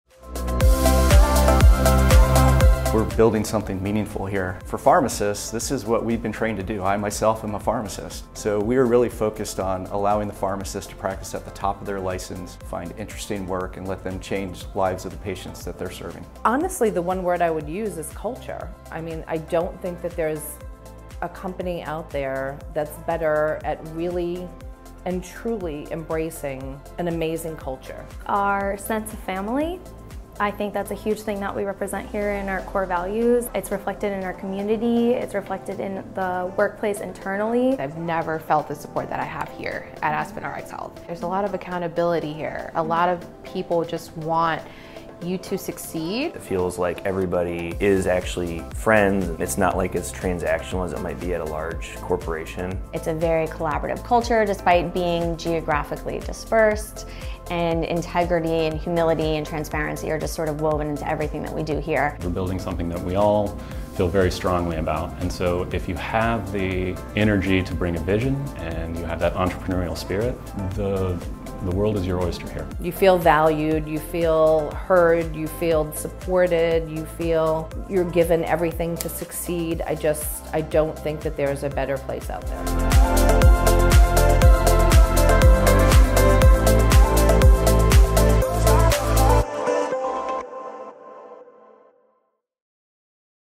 Aspen RxHealth Corporate Careers
Take an inside look at the passion and enthusiasm that Aspen RxHealth employees have as they share why they love working for the organization. Our dedicated team members explain firsthand the remarkable aspects that make Aspen RxHealth a truly exceptional workplace. From our vibrant culture to our unwavering mission of improving patient health through medication optimization, discover the deep sense of purpose that drives us.